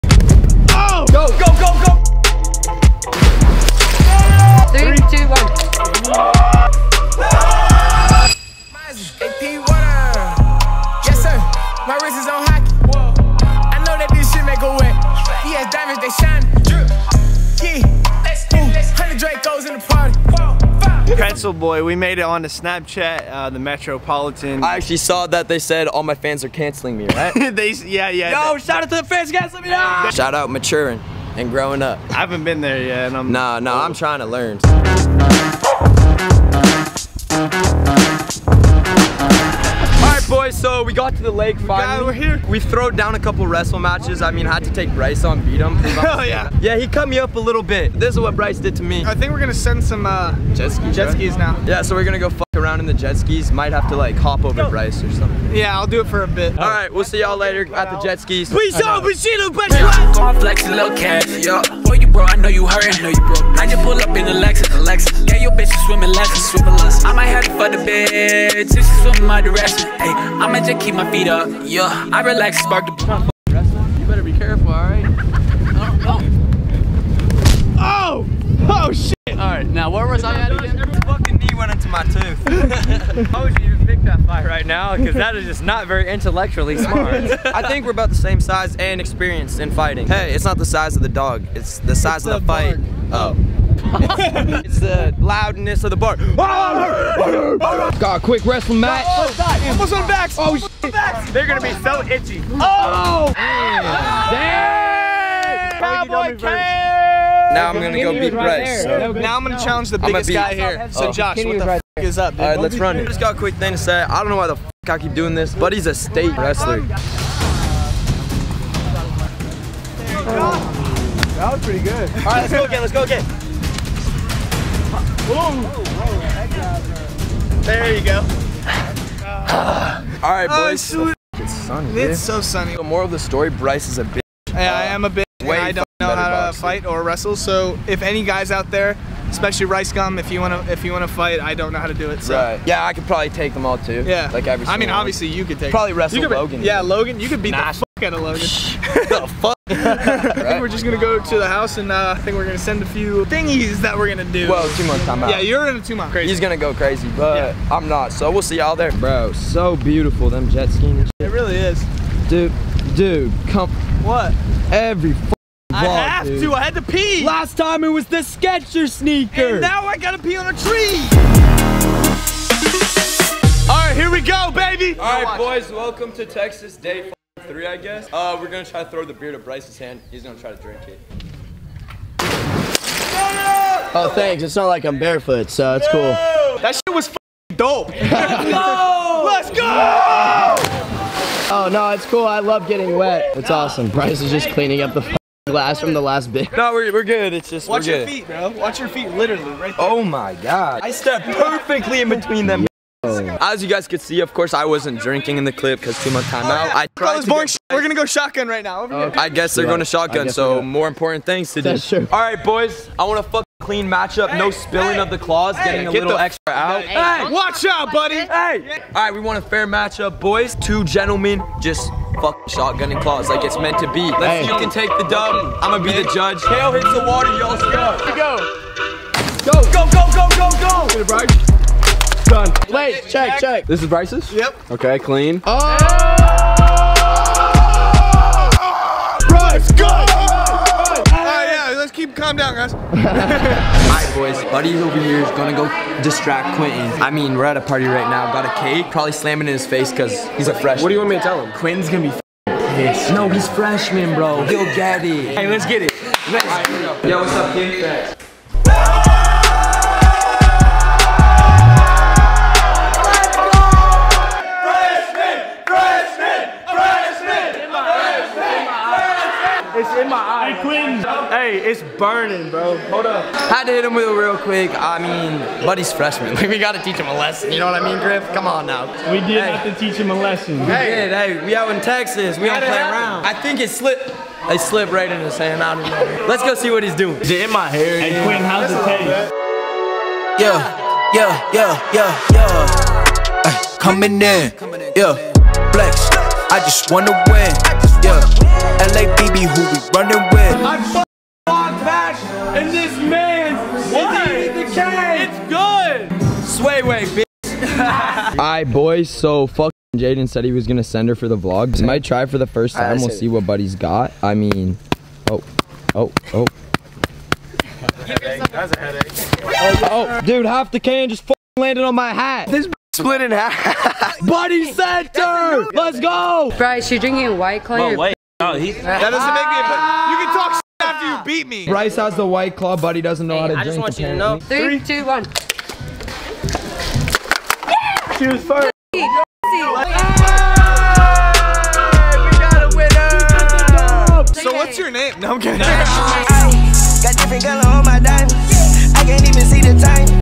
I Initiate Quinton Griggs Into The Sway House??!
Vlog 2/3 of the Texas, shit went wild so sit back, enjoy, and get ready for some crazy clips.

Edited by JROD: @officialjrod